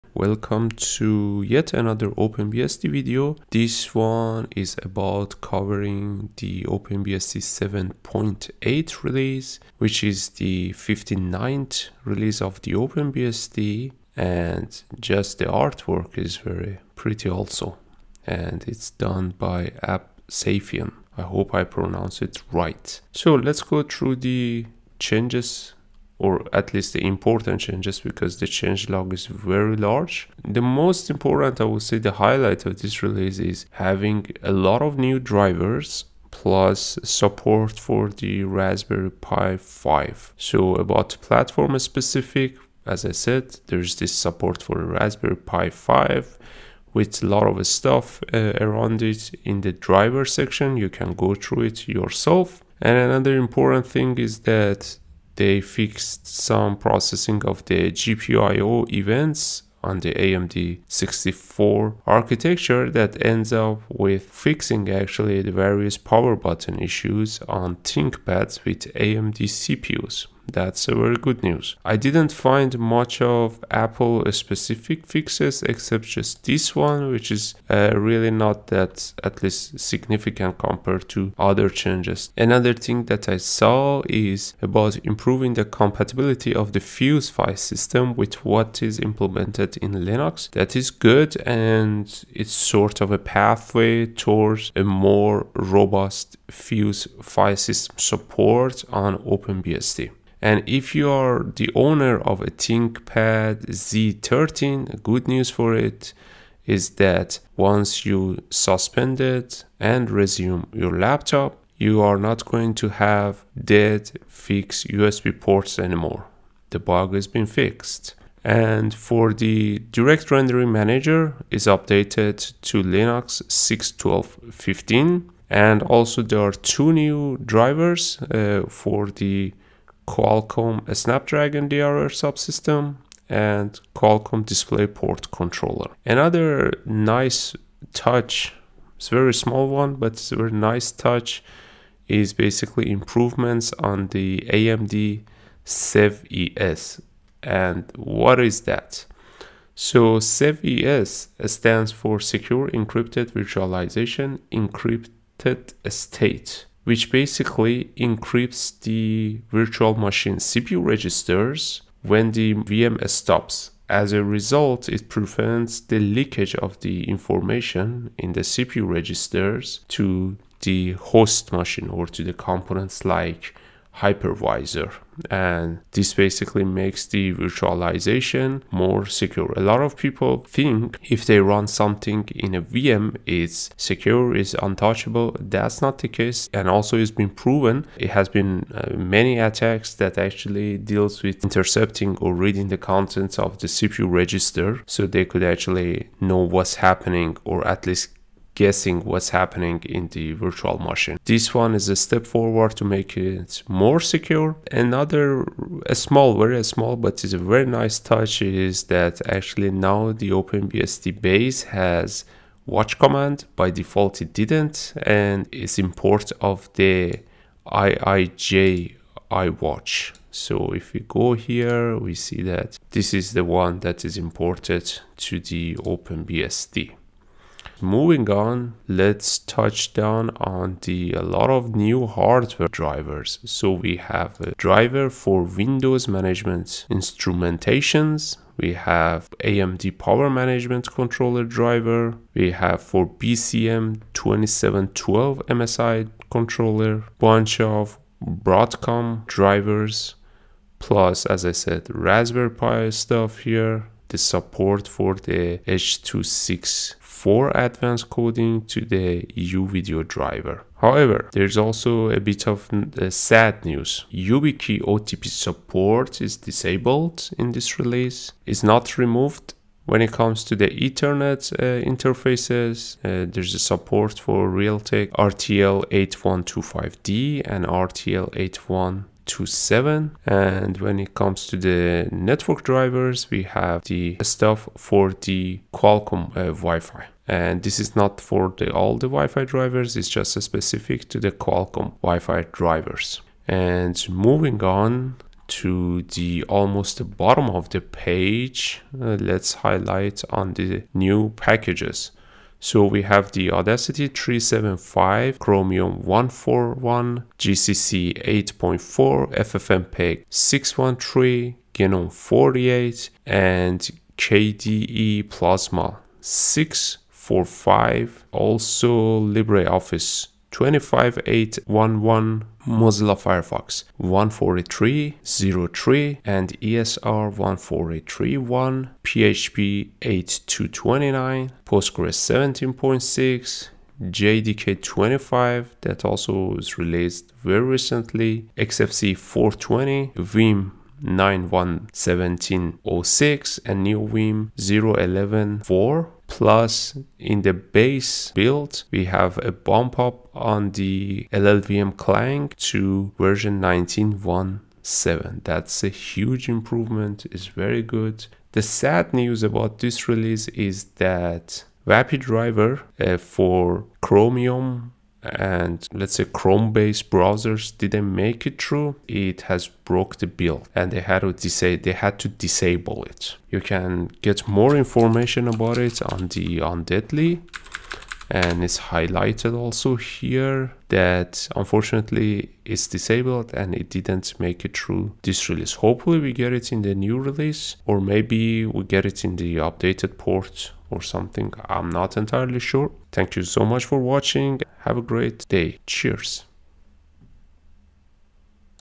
OpenBSD 7.8 is here (packed with new drivers)
Let's explore the highlights of this release of OpenBSD that includes many new drivers.